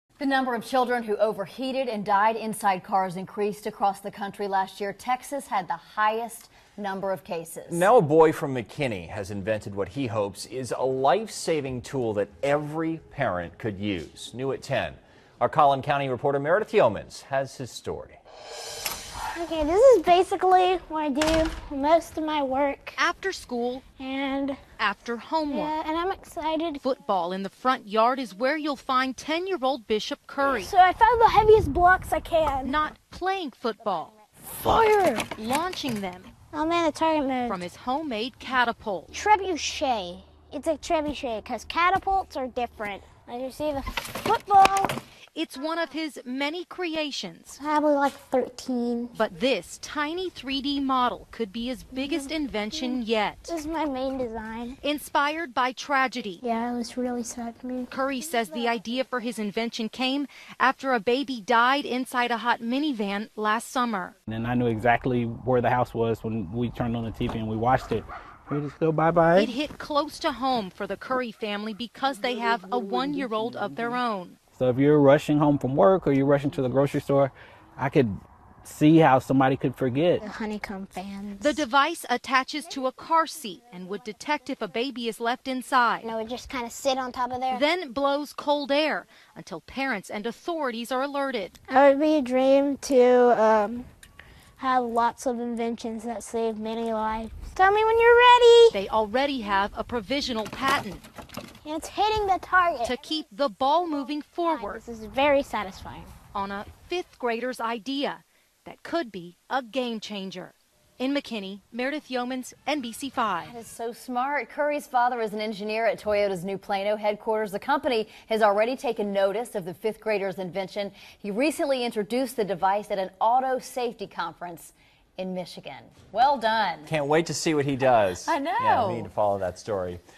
Young Genius 10 Year Old Boy Invents A Device To Prevent Babies From Dying In Hot Cars!
Plus d’AfrikaTech ? C’est par ici !